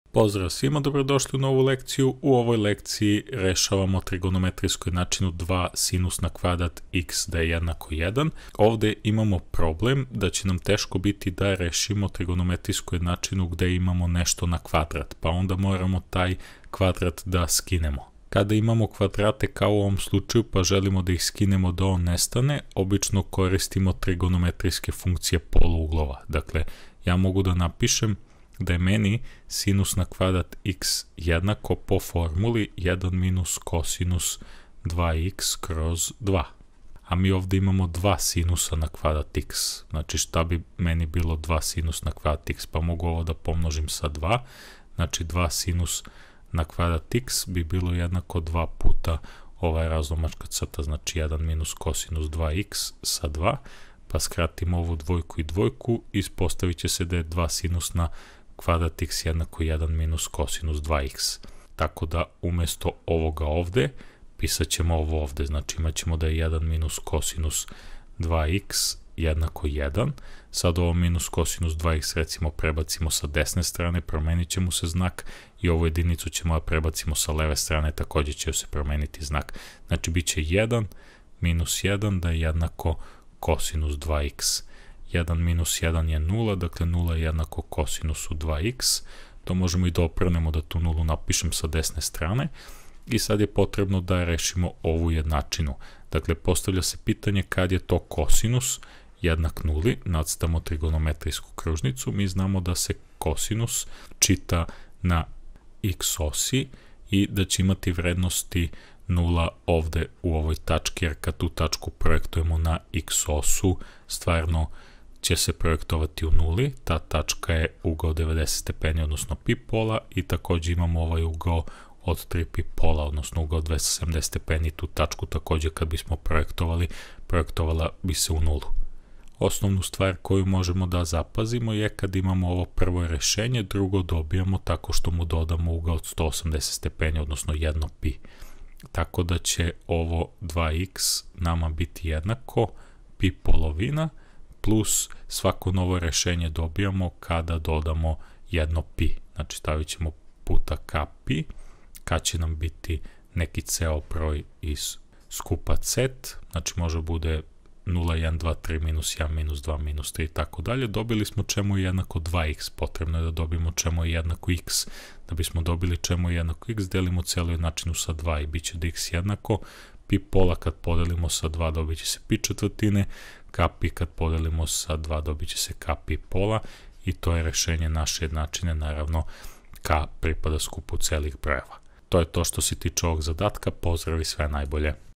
Trigonometrijske jednačine. Priprema za prijemni ispit na fakultetima
U ovoj lekciji rešavamo zadatak iz oblasti trigonometrijske jednačine. Priprema za prijemni ispit na fakultetima i višim školama.